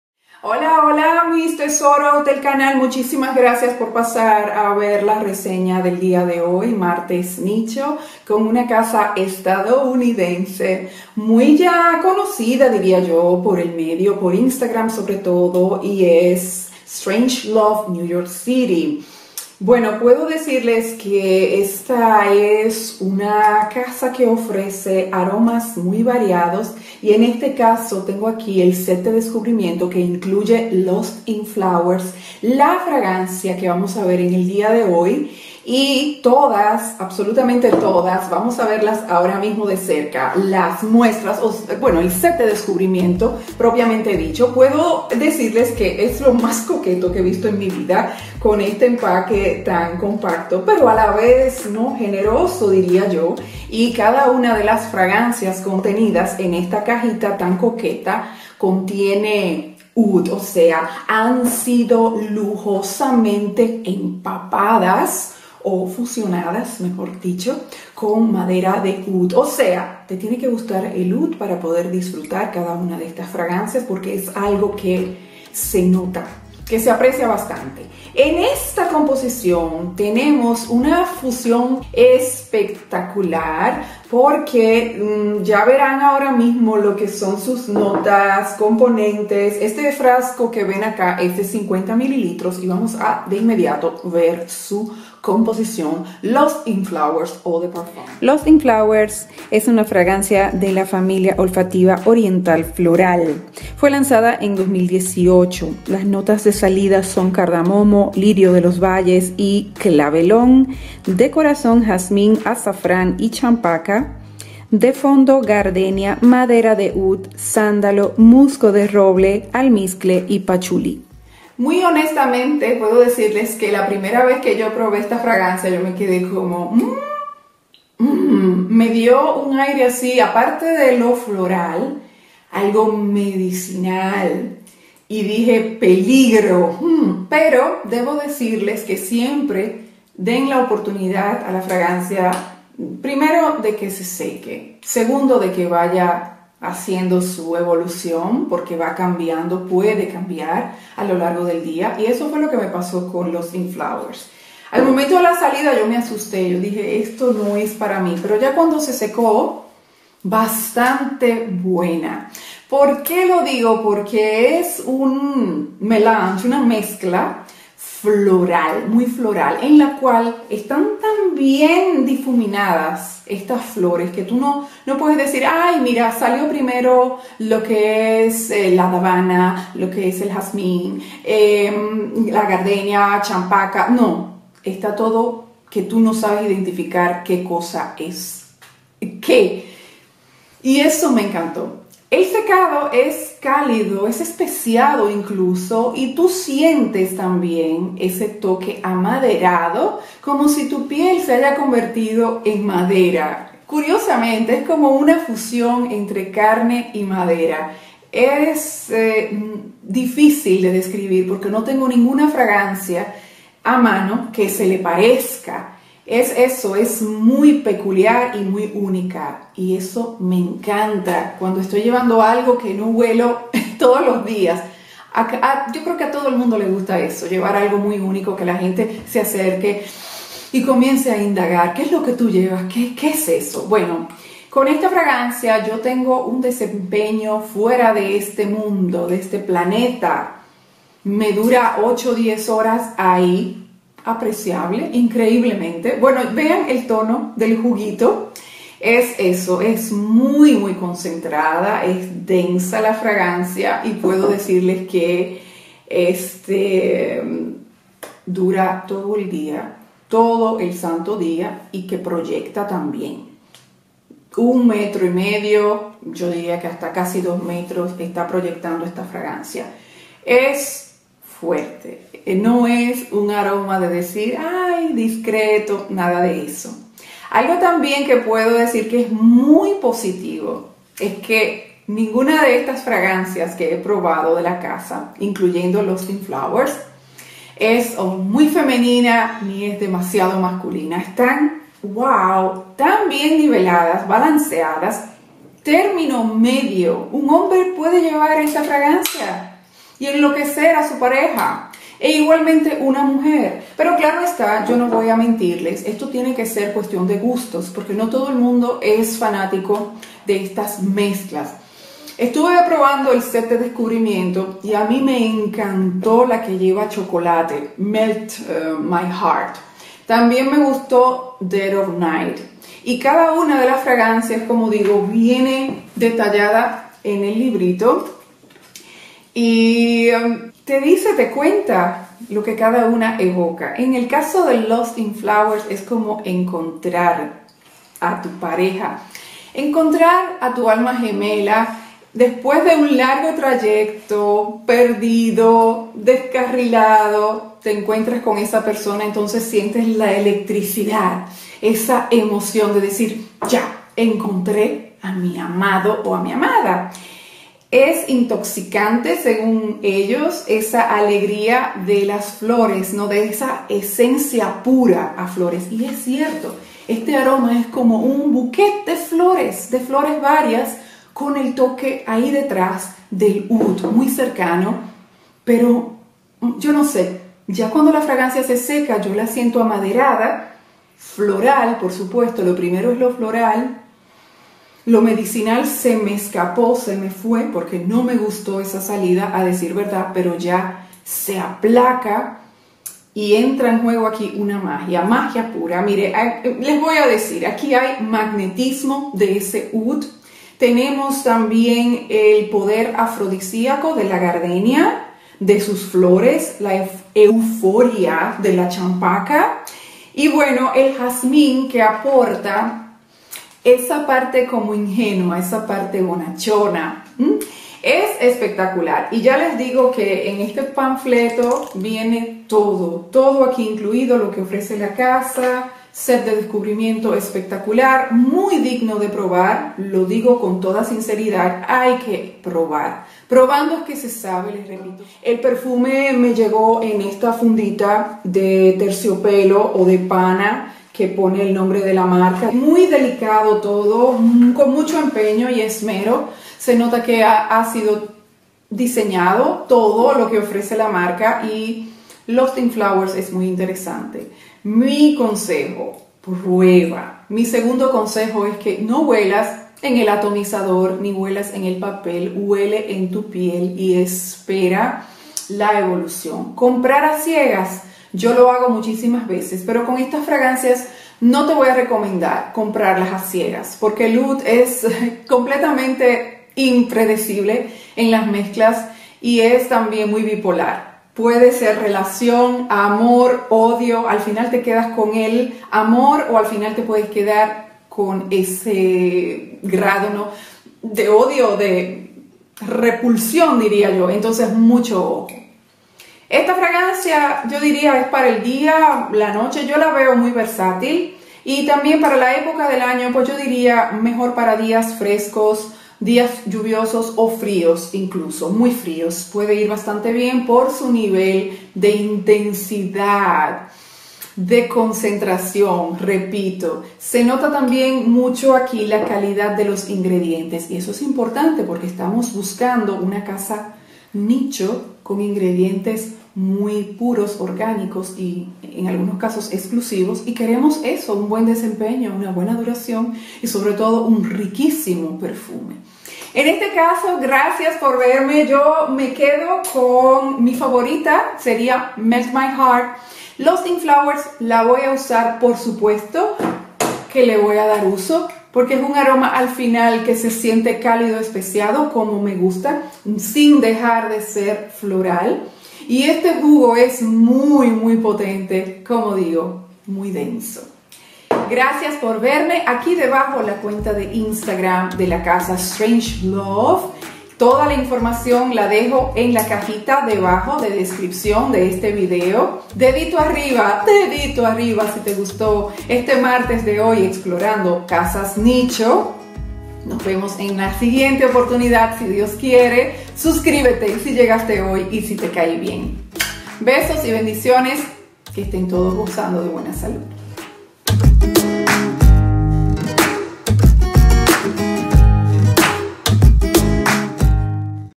STRANGE LOVE NYC. LOST IN FLOWERS REVIEW
Strange Love NYC es una casa de perfumería nicho creada en 2014, fue fundada por Elizabeth Gaynes. Su directora global es la modelo Helena Christensen. Hoy vamos a conocer la fragancia Lost in Flowers, Eau de Parfum. Oriental Floral de 2018. La nariz detrás de ella es Christophe Laudamiel.

📌 CÓDIGOS ESPECIALES 📌
- CAJITA DE SUSCRIPCION MENSUAL DESCUBRE PERFUMES NICHO
YUNIQU CODIGO 30% DEARMAKEUP

Karina Dilone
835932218
Packstation 103
76437, Rastatt
Germany.-